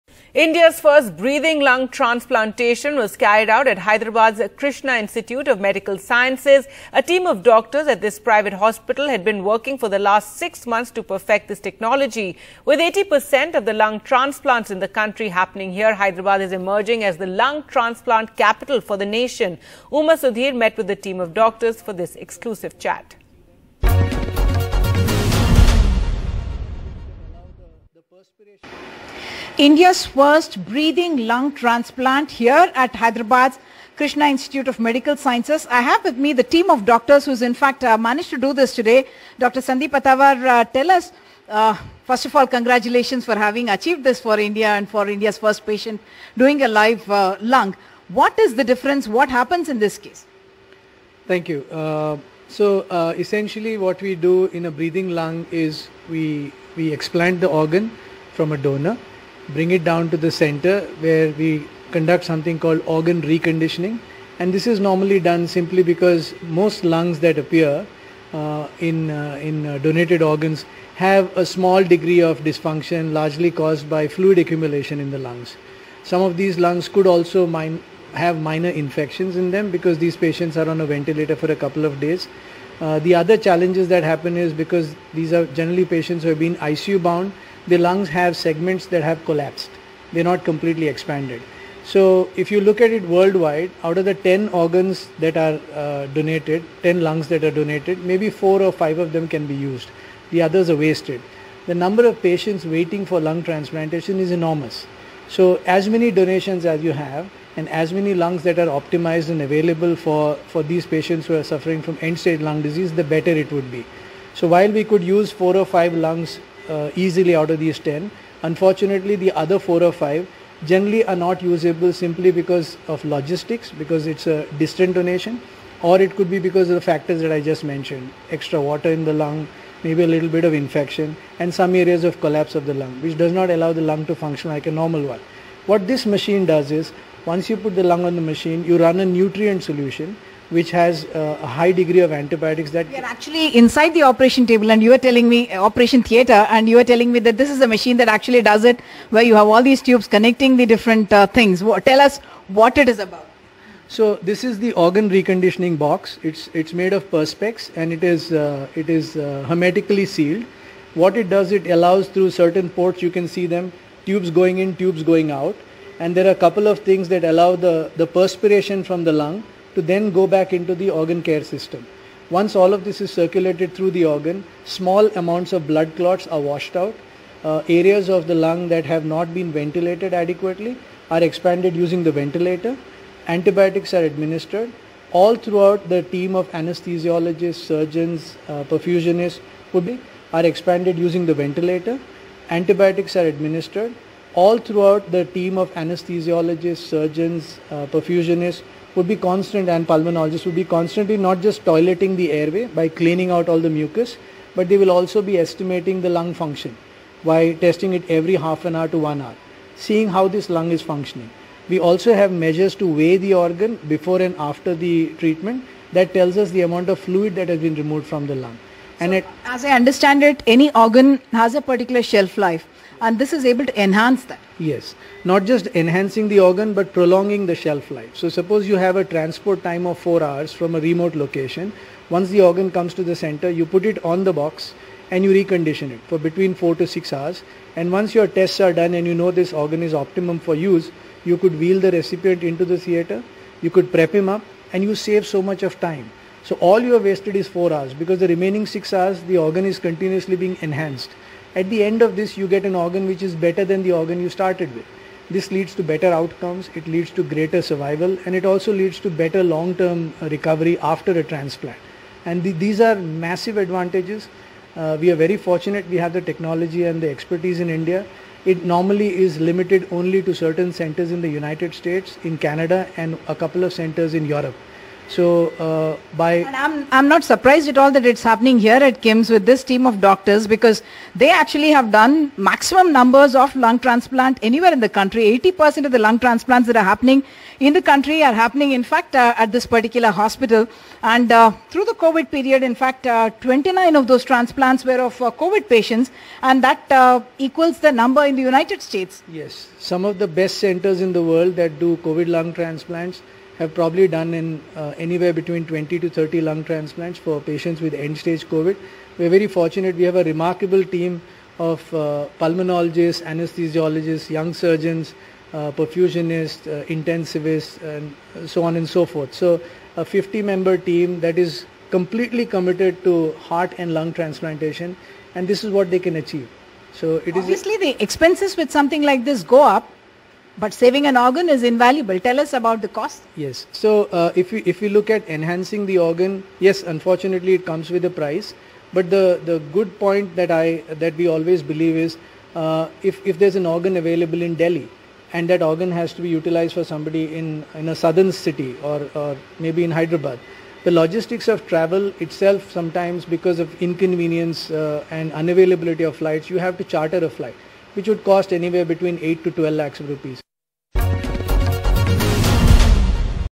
Hyderabad Sees India's First Cutting-Edge Lung Transplant
India has become one of a handful of nations including the US and Canada where a "breathing lung transplant" can be conducted - a huge advantage as there is a growing list of those waiting to get new lungs and Coronavirus only increased the critical cases of lung failure.